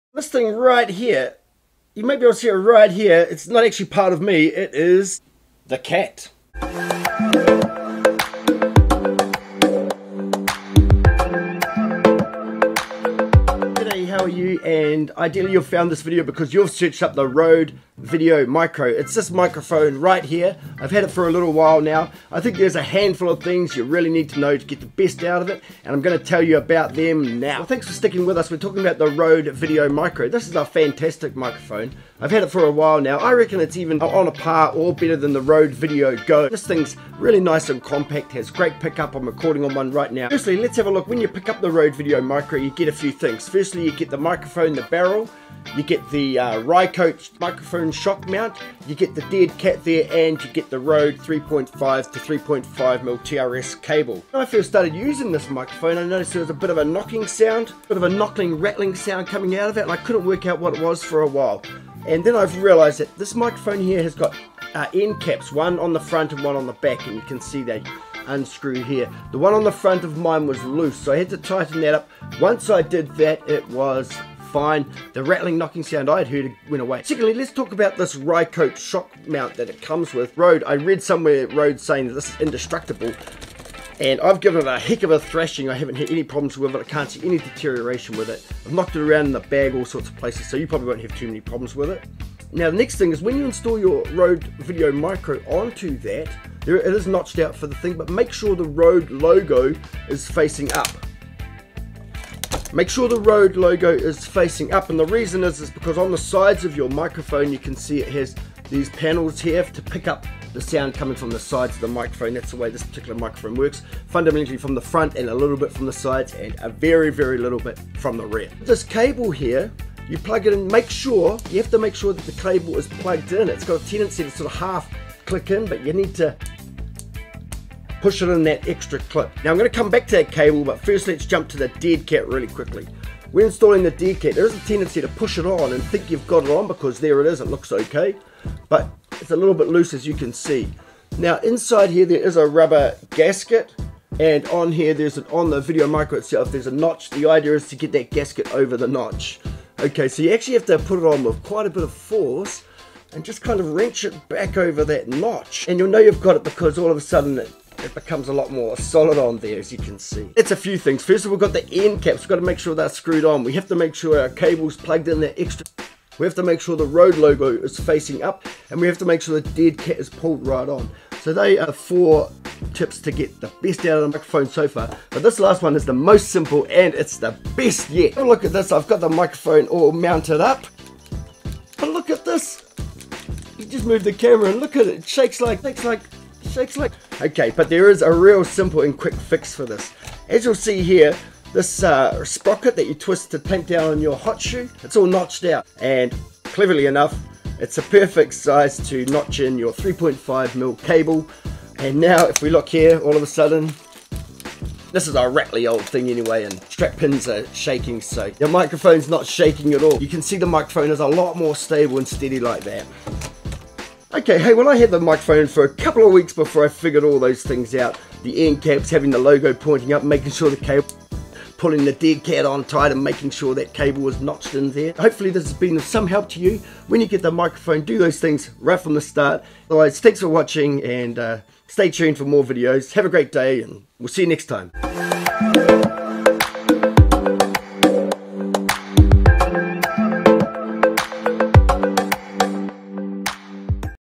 Get the best out of RODE VIDEO MICRO ~ follow these tips for perfect performance!
The Video Micro by Rode is a terrific external microphone with nice performance,  but there are a handful of things you need to know to get the best results from the microphone so check them out.